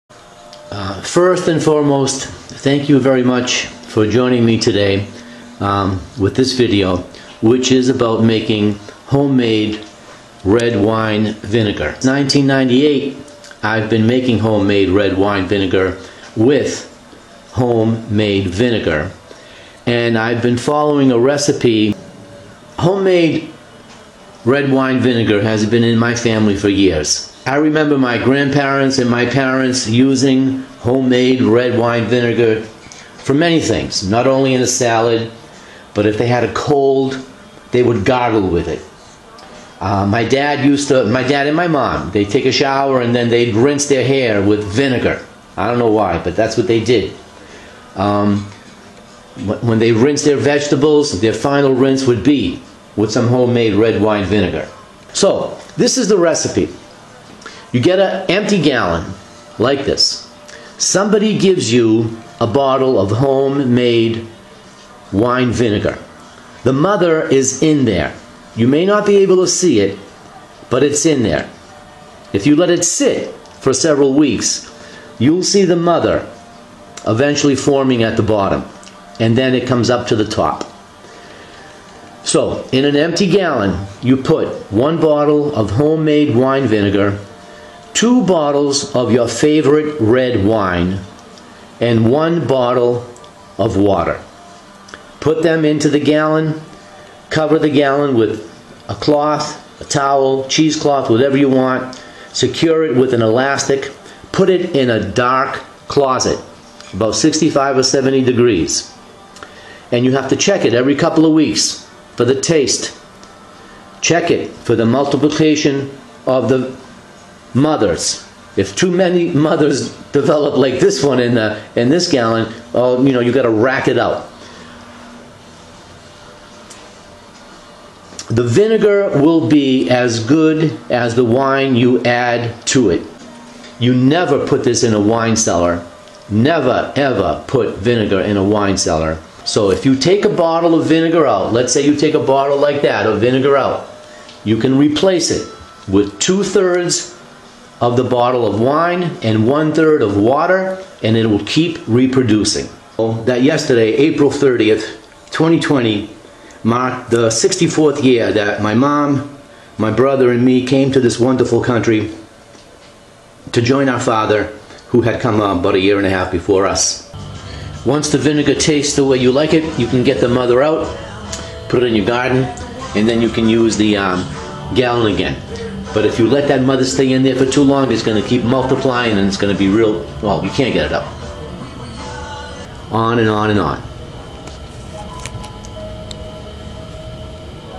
One way to make homemade red wine vinegar
Somebody gives you a bottle of homemade red wine vinegar (If you need a bottle, let me know.) and you follow this recipe for a never-ending vinegar supply = 1 bottle homemade vinegar, 2 bottles of wine, 1 bottle of water, all into a gallon, cover gallon with cloth, secure with elastic and place in dark closet for 3 months checking frequently for taste and to make sure the mother does not become too big. When it's ready, take a bottle out and replace with 2/3 wine and 1/3 water. Recipe comes from The Complete Handbook of Winemaking, with some modifications. From the same mother, you can make different kinds of vinegar depending on the wine you add: homemade or bought, the better the wine, the better the vinegar. Some information about how old timers used homemade red wine vinegar.